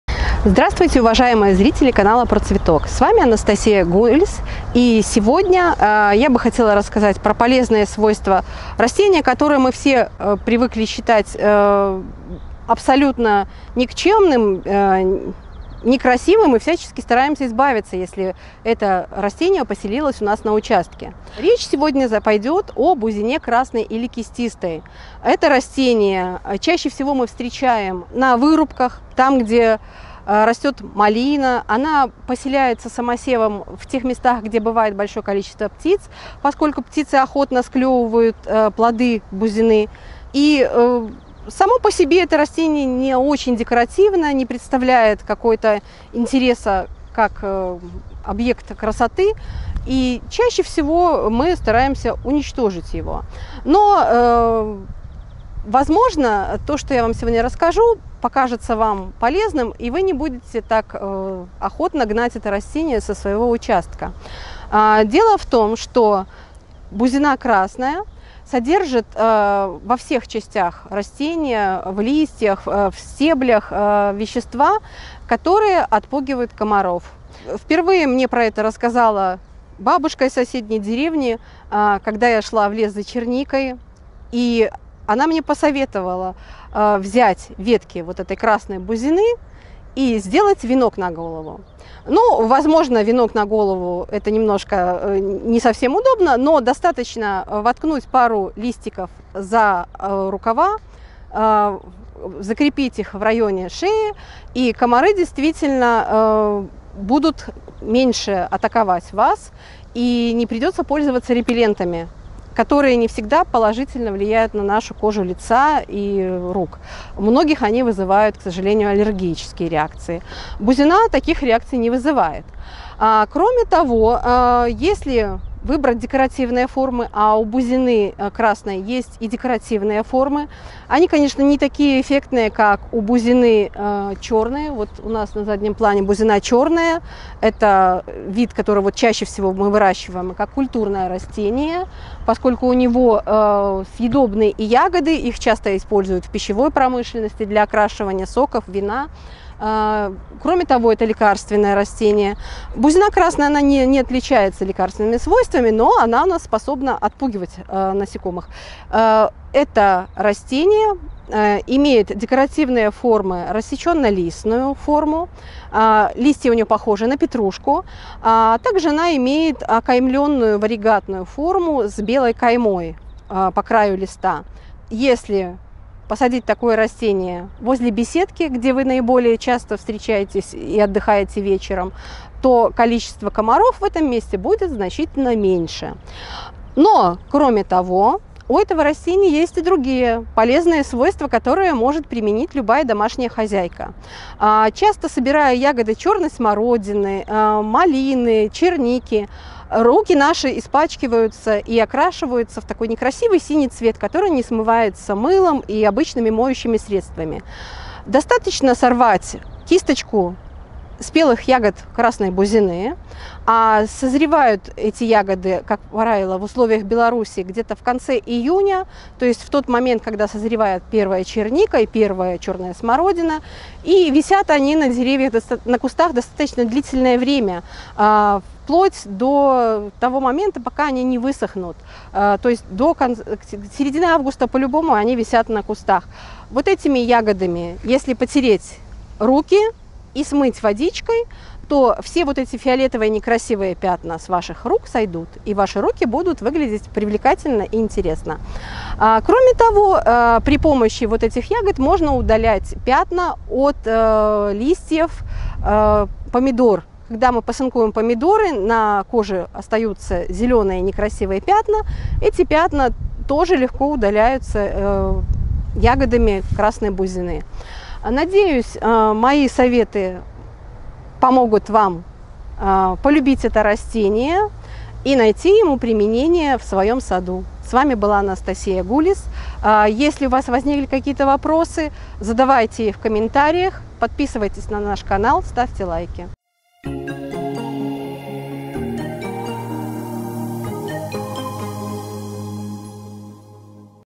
ЭТО РАСТЕНИЕ ПРОГОНИТ ВСЕХ КОМАРОВ С ВАШЕГО УЧАСТКА!
✿Всё для сада и огорода ищите в нашем интернет-магазине! ✔Беларусь ✔Россия ✔Казахстан ✔Украина ✔Задать вопрос, прочитать статью, поделиться фотографиями вы можете на

Как избавиться от комаров на участке? Я расскажу об одном проверенном народном средстве. Вам понадобится посадить на участке всего одно растение, и комары исчезнут навсегда.